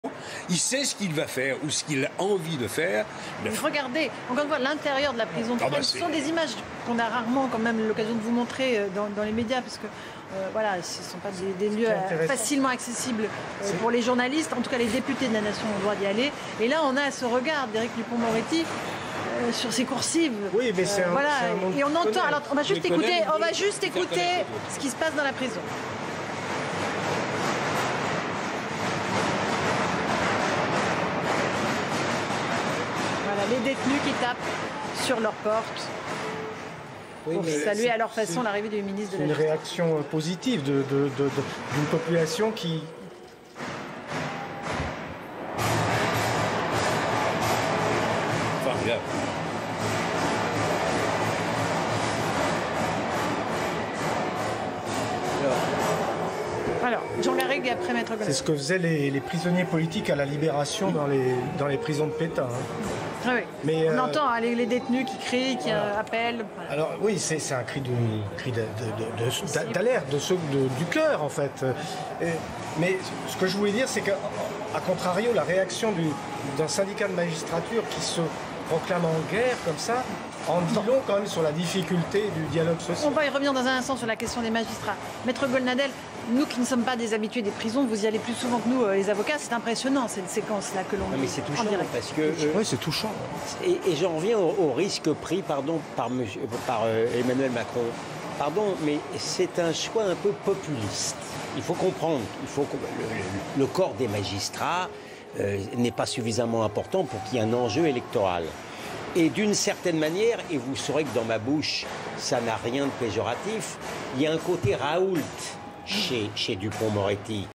Le cri du cœur des détenus de la prison de Fresnes lors de la visite d'Éric Dupond-Moretti
Les détenus de la prison de Fresnes ont bruyamment salué l'arrivée d'Éric Dupond-Moretti à la prison de Fresnes, ce mardi 7 juillet.